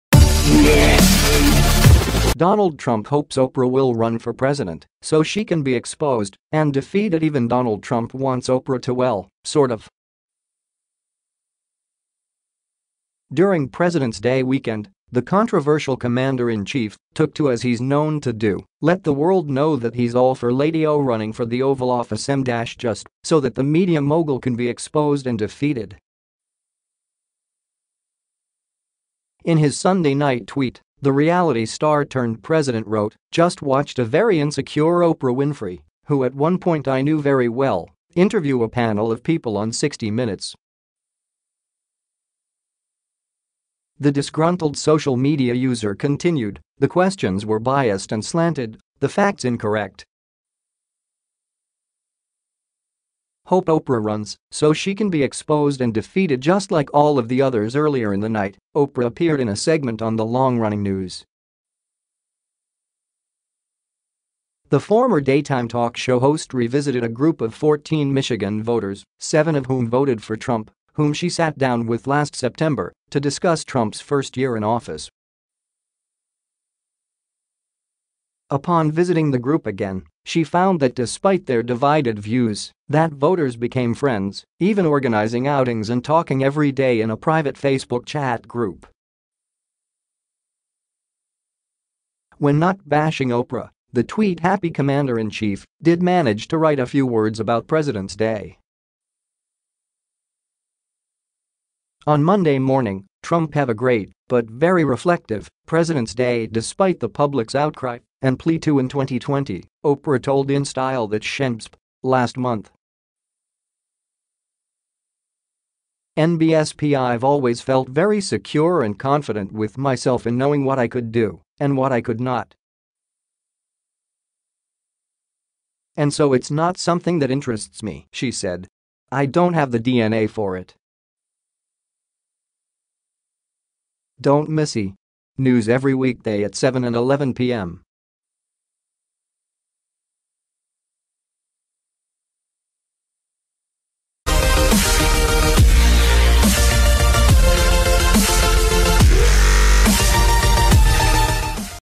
Donald Trump Hopes Oprah Will Run for President So She Can Be ''Exposed and Defeated''
Donald Trump Hopes Oprah Will Run for President So She Can Be ''Exposed and Defeated''  |   Even Donald Trump wants Oprah to ! Well, sort of...During President's Day weekend, the controversial commander-in-chief took to , as he's known to do, let the world know that he's all for Lady O running for the oval officemdash;just so that the media mogul "can be exposed and defeated."In his Sunday night tweet, the reality-star-turned president wrote, "Just watched a very insecure Oprah Winfrey, who at one point I knew very well, interview a panel of people on 60 Minutes."The disgruntled social media user continued, "The questions were biased and slanted, the facts incorrect. Hope Oprah runs so she can be exposed and defeated just like all of the others!"Earlier in the night, Oprah appeared in a segment on the long-running news . The former daytime talk-show host revisited a group of 14 Michigan voters, seven of whom voted for Trump, whom she sat down with last September to discuss Trump's first year in office.Upon visiting the group again, she found that despite their divided views that voters became friends, even organizing outings and talking every day in a private Facebook chat group.   When not bashing Oprah, the tweet-happy commander in chief did manage to write a few words about President's Day.On Monday morning, Trump , "Have a great, but very reflective, President's Day!"Despite the public's outcry and plea to  in 2020, Oprah told InStyle  that shenbsp; last month.nbsp;"I've always felt very secure and confident with myself in knowing what I could do and what I could not. And so it's not something that interests me," she said. "I don't have the DNA for it."   Don't miss E! News every weekday at 7 and 11 p.m.